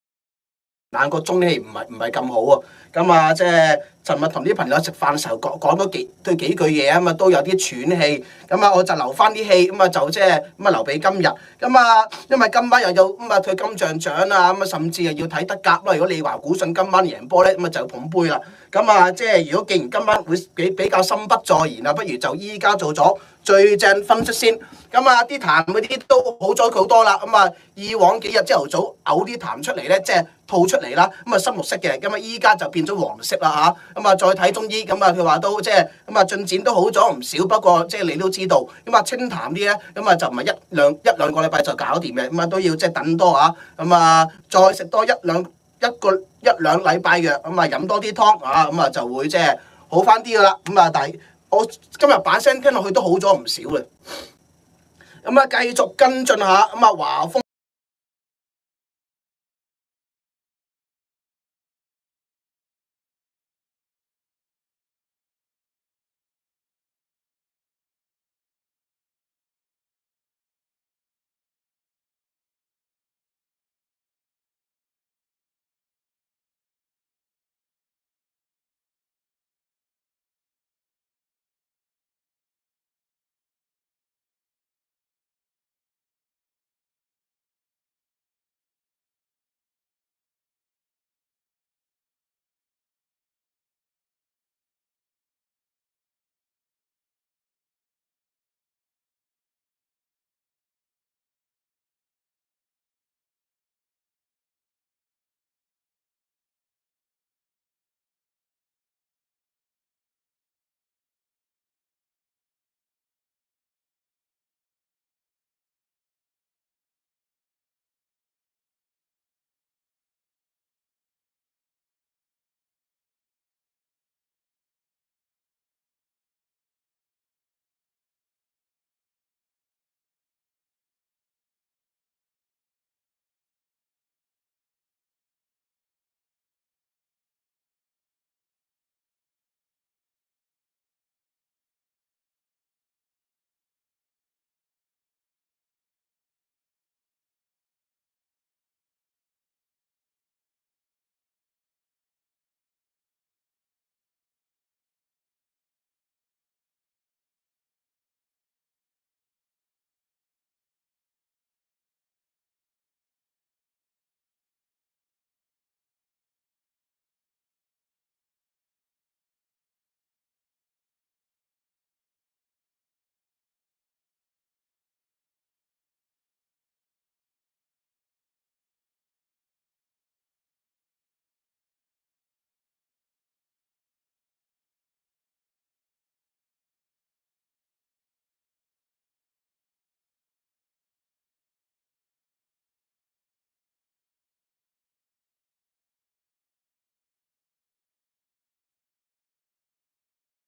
20240414 最正分析打女人偷錢賊林匡正又偷相鳩吹13號星期六晚死咗去同啲仆街去咗坐鎮食飯，又吹病到仆街唔夠氣要食多兩個禮拜藥博同情。病到咁仲食咁多煎炸嘢？唔夠氣可以連講都幾秒嘢唔洗吸氣？去咗葵廣
買咗兩杯嘢又影相出帖，一日又去葵廣又去土瓜灣，去土瓜灣食飯但碟碟嘢都咁近佢嚟影相，啲仆街嘢食要擺係佢面前先擺去食嗰條仆街面前？之前定之後死咗去葵涌？完全唔合邏輯
係吹唔到坐鎮啲嘢食幾錢

吹到咁大先呃到5-6件腦殘＋姣闉 388蚊

連點為之唔夠氣都唔識，自曝其醜
吹乜撚嘢要睇金像奬早啲做費時心掛掛？
學埋閪人歐陽永權妄想自己好撚重要，啲腦殘等睇
做完個節目 43分鐘都係得 1700個views
而家啲腦殘都唔理佢做唔做，而佢根本因為呃錢唔到而唔撚想做
之前唔係吹撚到星期六日，公眾假期要陪屋企人去食飯咩？
星期六唔係去睇波就吹同啲仆街去食飯，偷相就偷相啦，扮有錢去應酬
星期日就鳩吹要睇金像奬扮忙
靚煲皇食到 15號又無咗一間，徐記廚房又無埋，真係要落力啲去搵新
黏咗靚煲皇超過十年免費餐，最後一個月都唔帶啲仆街去撐埋佢
就睇到偷錢賊有幾撚反骨
去靚煲皇撐場自己唔洗，但啲仆街要俾，又黏餐又會俾人講，無錢俾咪要偷相吹同啲仆街去食第二間扮有錢仔囉